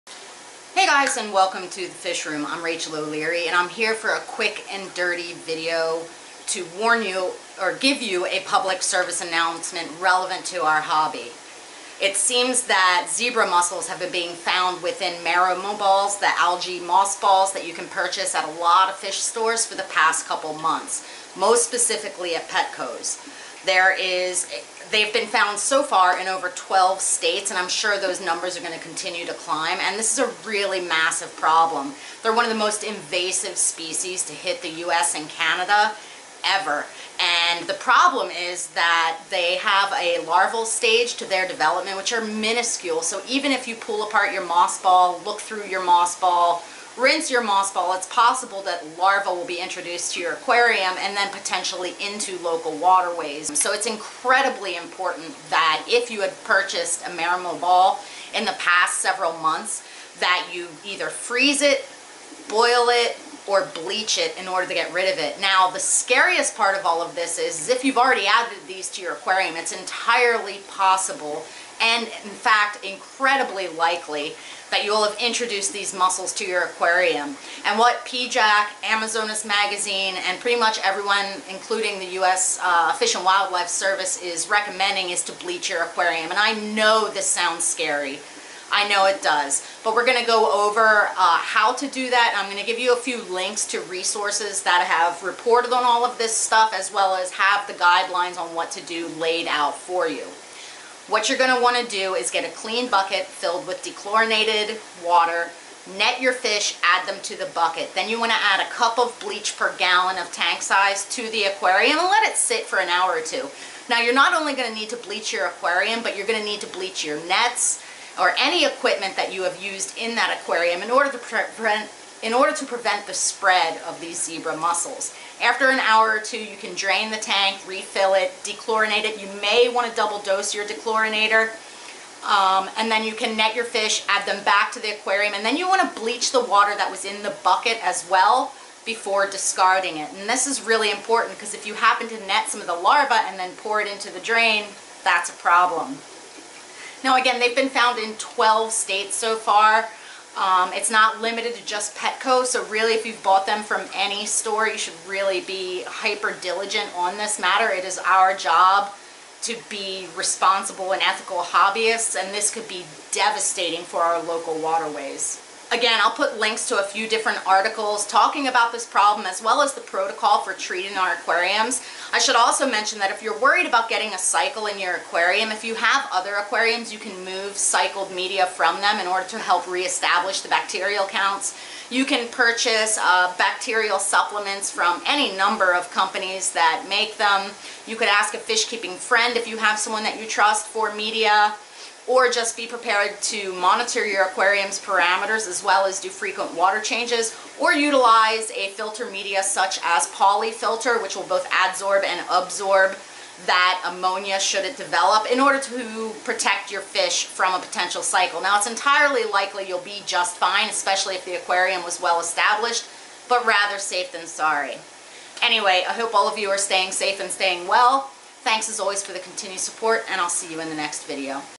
Fish Stores selling contaminated moss balls??! Beware Invasives!
The count is up to 21 States affected by the contaminated moss balls. Make sure to buy 100% bleach- no added fragrances or surfactants.

How to sterilize:
DISPOSE of the moss ball and any of its packaging in a sealed plastic bag in the trash. If vinegar, boiling water, or bleach was used, the liquid can be disposed down a household drain —never down a storm drain where it could enter and damage local waterways.

DRAIN and clean the aquarium.

Collect any fish or other living organisms and place them in another container, with water from a separate, uncontaminated water source.  Sterilize the contaminated aquarium water by adding ¼ teaspoon bleach for each gallon of water. Let the water sit for 10 minutes and then dispose the sterilized water down a household drain.

Clean the aquarium and accessories using one of the following methods, ensuring that the decontaminate method you choose is in accordance with manufacturers’ recommendations:

    Hot Water Method:
        Use water that is 140 degrees F to flush and coat the tank and all accessory surfaces for at least one minute, OR
    Disinfection Method:
        Submerge the moss ball in 1 cup of chlorine bleach per gallon of water for 10 minutes or undiluted white vinegar for 20 minutes.
        Soak the aquarium, substrate, rocks, décor, and filter media in the bleach water solution for 10 minutes.
        Rinse off all items prior to setting up the aquarium.
        Dispose of the previously used filter media and replace with new media.
        Use a dechlorinating product to neutralize any residual chlorine prior to reintroducing aquatic life.